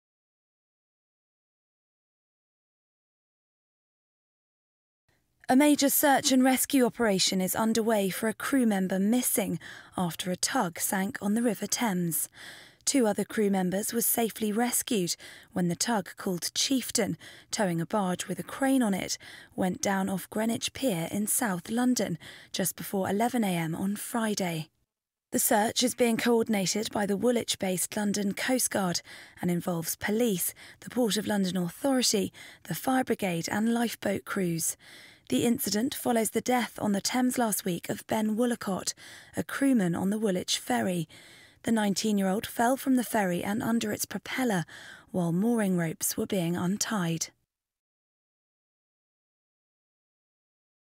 Crew member missing in Thames
A major search and rescue operation is under way for a crew member missing after a tug capsized and sank on the River Thames.